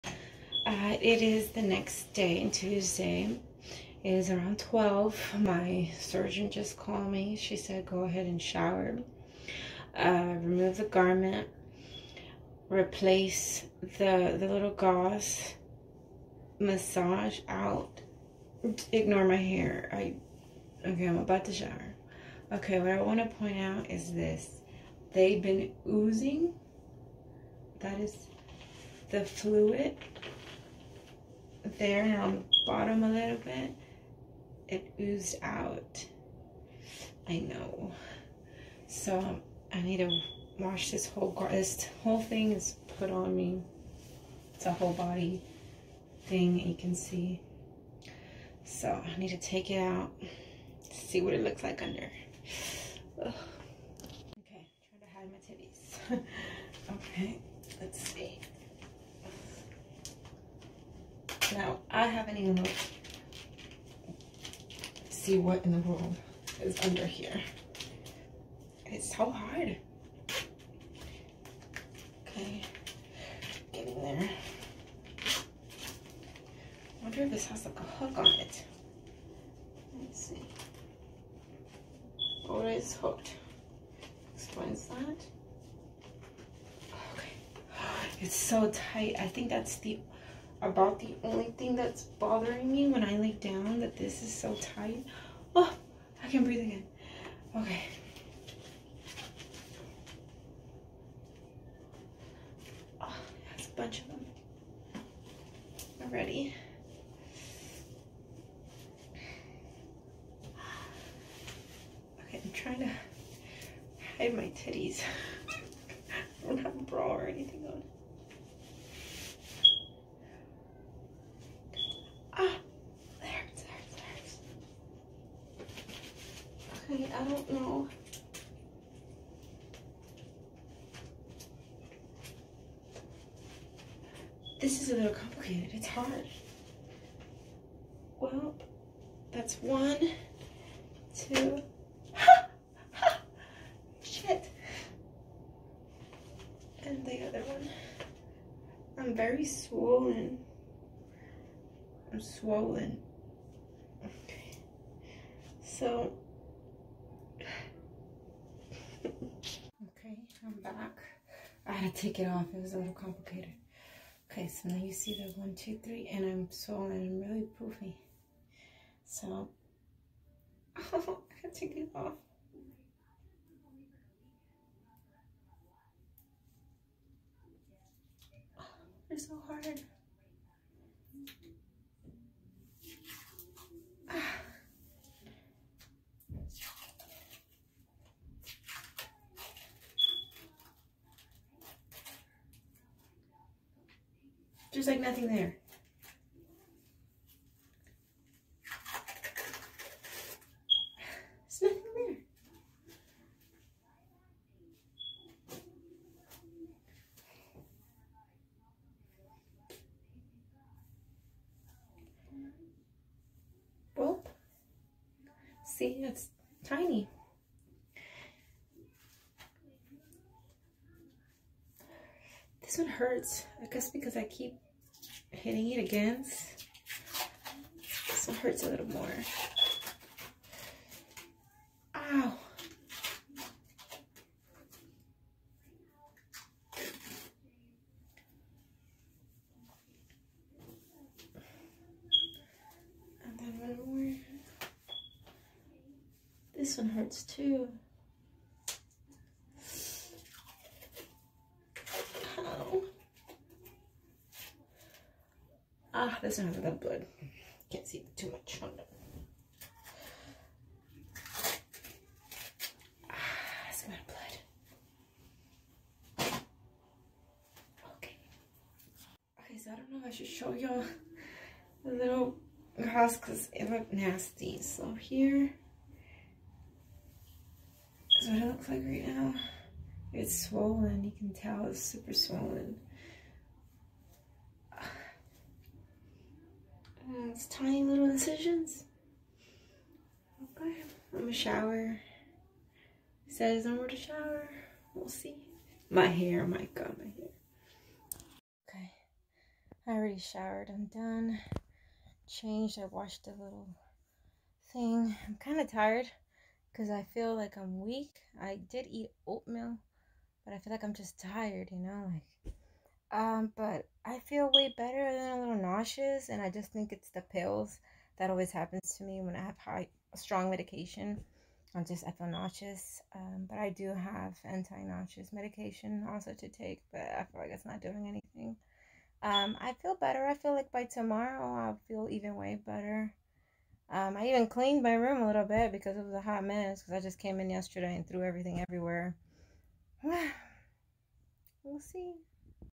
"Warning Graphic"😷 🤢 Smart Lipo Recovery Day 1
Warning Graphic you will see blood!

July 30 Day after procedure day one of recovery from Smart Lipo . Still in pain